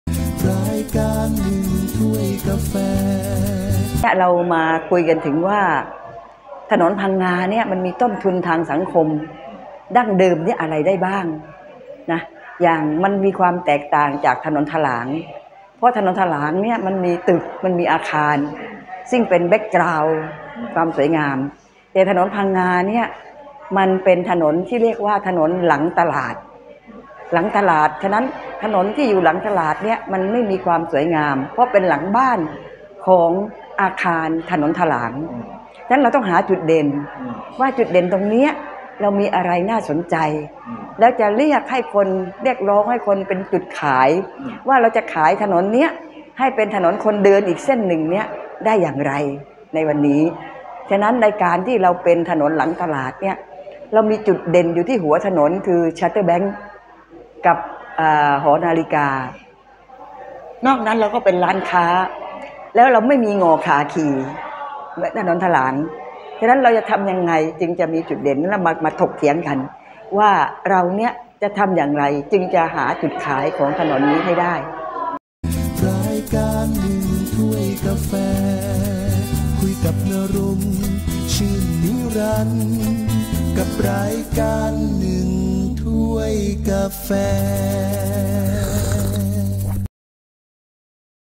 หาจุดเด่น ของถนนพังงา เพื่อจัดให้เป็นถนนคนเดิน
By.ณรงค์  ชื่นนิรันดร์
           ครูฤดี  ภูมิภูถาวร นักประวัติศาสตร์ภูเก็ต  บอกว่า  เรามาคุยกันว่าถนนพังงา  มีต้นทุนทางสังคม  ดั้งเดิมอะไรบ้าง  ถนนพังงามีความแตกต่างจากถนนถลาง  เพราะถนนถลางมีตึก มีอาคาร  ซึ่งเป็น background ให้ความสวยงาม  แต่ถนนพังงาเป็นถนนที่เรียกว่า  ถนนหลังตลาด  ดังนั้นถนนหลังตลาดจึงไม่ค่อยมีความสวยงาม  เพราะเป็นหลังบ้านของอาคารในถนนถลาง  ดังนั้นจึงต้องหาจุดเด่น
           จุดเด่นของถนนพังงา  มีอะไรบ้างที่น่าสนใจ  แล้วจะสร้างให้เป็นจุดขาย  ให้ถนนพังงาเป็นถนนคนเดิน ได้อย่างไร  การที่ถนนพังงาเป็นถนนหลังตลาด  แต่เรามีจุดเด่นที่หัวถนนคือ  ชาร์เตอร์ แบงค์  กลับหอนาฬิกา  นอกนั้นก็จะเป็นร้านค้า  แล้วถนนพังงา  ก็ไม่มี หง่อคาขี่  แล้วจะทำอย่างไรให้ถนนพังงามีจุดเด่น  จึงเป็นที่มาของการถกเถียงกัน  ว่าเราจะทำอย่างไร  จึงจะหาจุดขายของถนนพังงาได้

Find the distinctive points of Phang Nga Road to make it a walking street.
By.Narong Chuenniran
           Teacher Ruedee Phumphuthaworn, Phuket historian, said that let's talk about Phang Nga Road. There is a social cost. What are the traditions? Phang Nga Road is different from Thalang Road. Because Thalang Road has buildings and buildings which serve as a beautiful background. But Phang Nga Road is a road called Lang Market Road, so the roads behind the market are not very beautiful. Because it is behind the house of the building on Thalang Road. There fore, you must find the highlight
         Highlights of Phang Nga Road What's interesting? Then it will be created as a selling point. How can Phang Nga Road be a walking street? The fact that Phang Nga Road is a road behind the market But we have a prominent feature at the head of the road, namely the Charter Bank, back to the clock tower. Other than that, there are shops. And Phang Nga Road doesn't have any places to ride. So what can we do to make Phang Nga Road have a distinctive pointTherefore it is a source of debate. What will we do? So you can find a selling point on Phang Nga Road